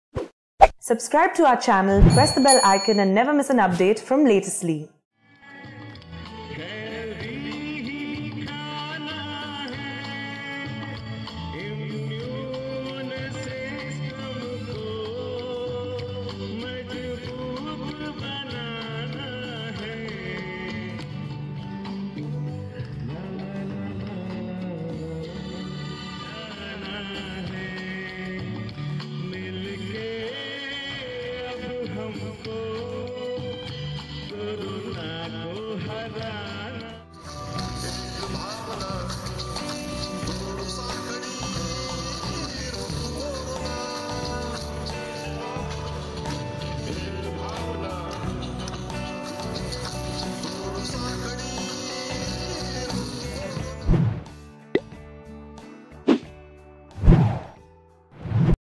Coronavirus: कोरोनाबद्दल जन जागृति निर्माण करण्यासाठी पोलिसांनी लढवली अनोखी शक्कल
नागरिकांमध्ये  कोरोना संदर्भात जागरूकता पसरवण्यासाठी पोलिसांनी वेगळा मार्ग निवडला आहे.गाणं म्हणून दाखवत सध्या पोलिस नागरिकांना आवाहन करताना पहायला मिळत आहे.